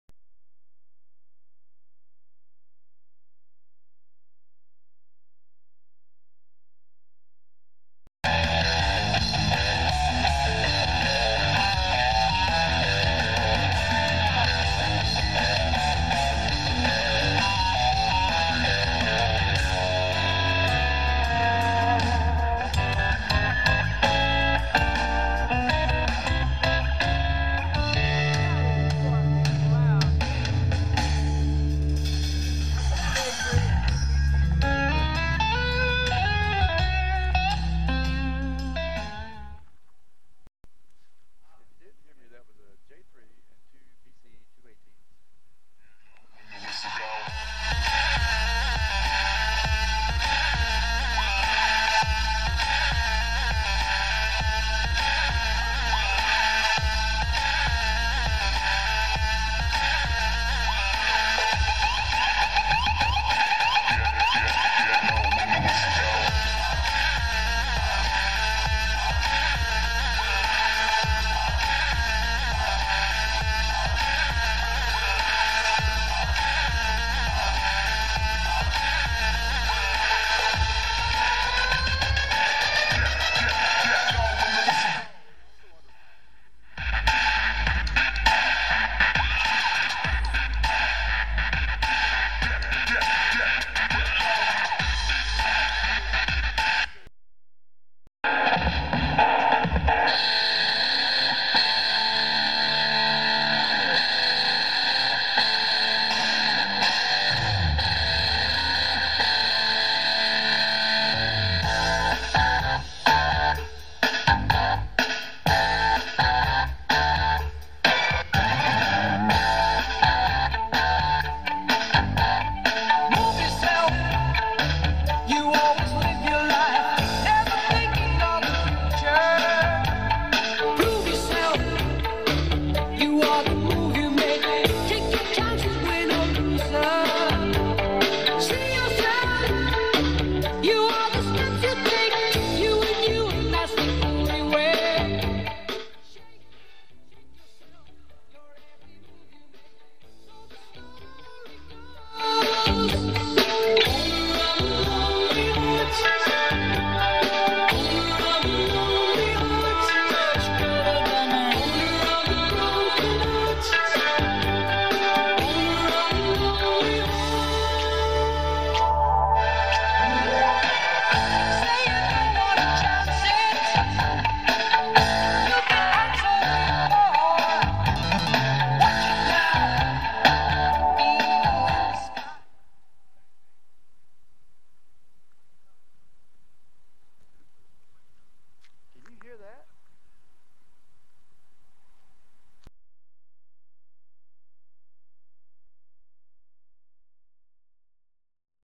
Danley Sound Labs BC218 and
an A-B-A demo of a J3 and a Caleb.